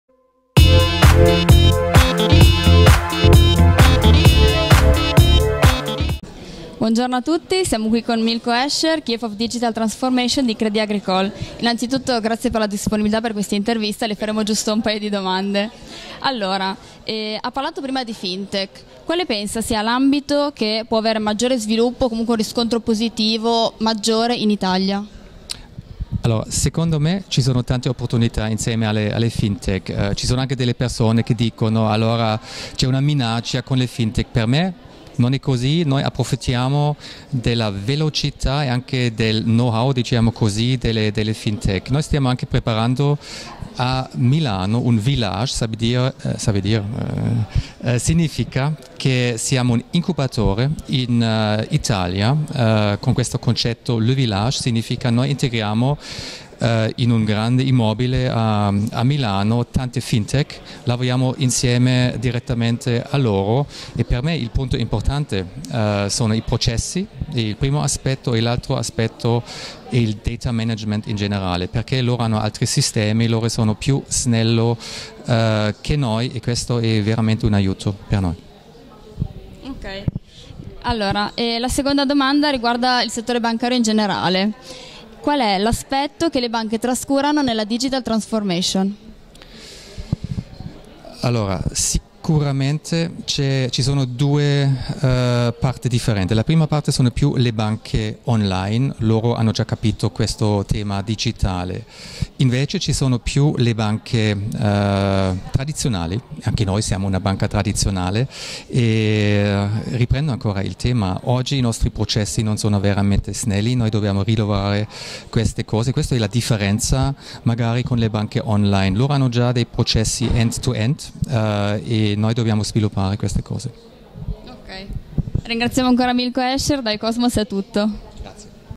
OIKOSMOS INTERVISTA MILKO HASCHER - CREDIT AGRICOLE
Digital Transformation e Fintech: scoprite con noi l'evoluzione del settore bancario!
Abbiamo intervistato Milko Hascher, Chief of Digital Transformation di Crédit Agricole